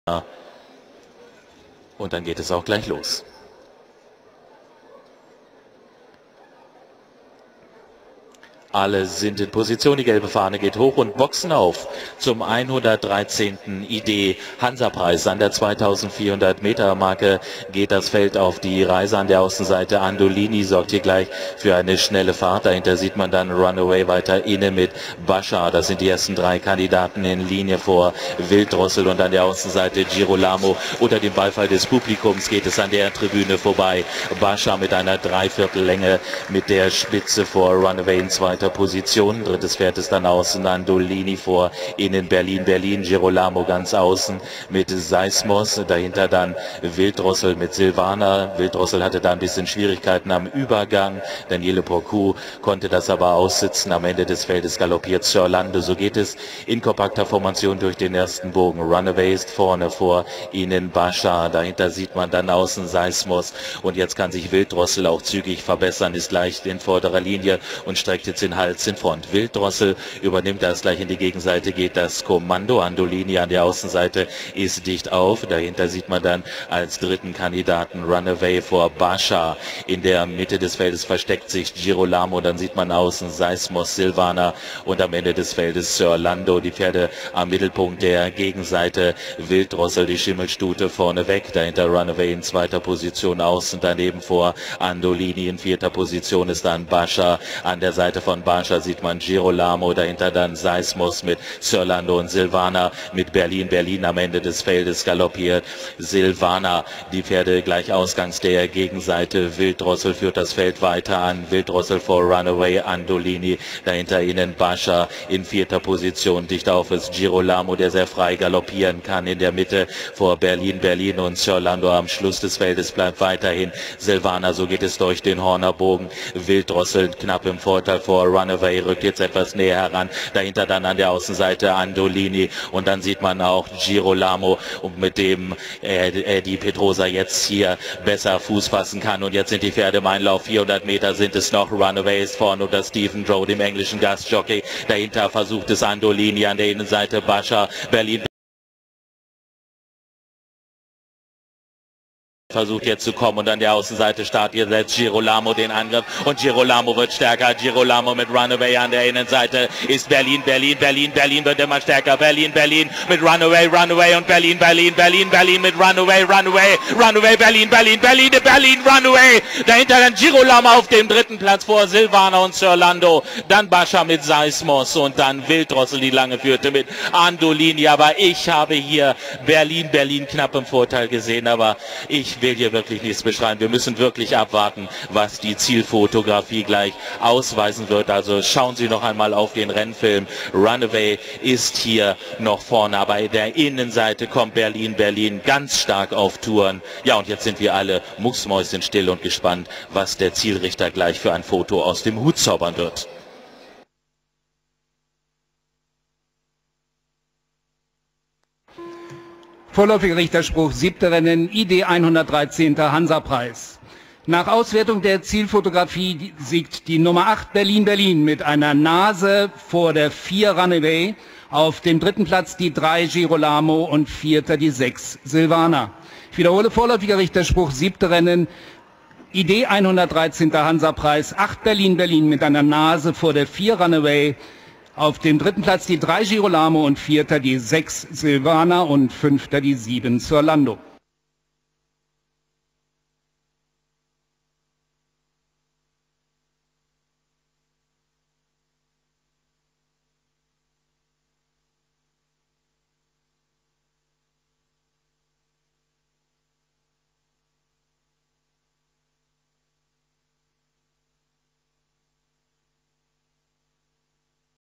Hamburg: IDEE Hansa-Preis (Gr. 2) - Siegerin: Berlin Berlin
IDEE Hansa-Preis Gruppe II (A) Preise: 70.000 € (40.000, 15.500, 6.500, 4.000, 2.500, 1.500). Ehrenpreis dem Besitzer, Andenken dem Trainer und Reiter des Siegers, gegeben von der J.J.Darboven GmbH & Co.KG. 1.400 € Einsatz (350, 490, 560). Für 3-jährige und ältere Pferde. Gew. 53,5 kg. f.3j., 60,0 kg. f.4j.u.ält. 2400m

1. Berlin Berlin, 58,5kg, Besitzer: Gestüt Görlsdorf, Trainer: Markus Klug, Jockey: A.Helfenbein
2. Runaway, 60,0kg, Besitzer: Gestüt Weiherwiesen, Trainer: A.Trybuhl, Jockey: S.Drowne
3. Girolamo, 60,0kg, Besitzer: Gestüt Ebbesloh, Trainer: P.Schiergen, Jockey: E.Pedroza

Ka. N-1¼-1½-H-2½-1¼-3½-¾ 2:33,62 Siegwette 230:10. - Platzwette 26, 17, 11:10. - Zweierwette 2.514:10. - Dreierwette 9.611:10. - Platz-Zwilling-Wette 200, 77, 25:10.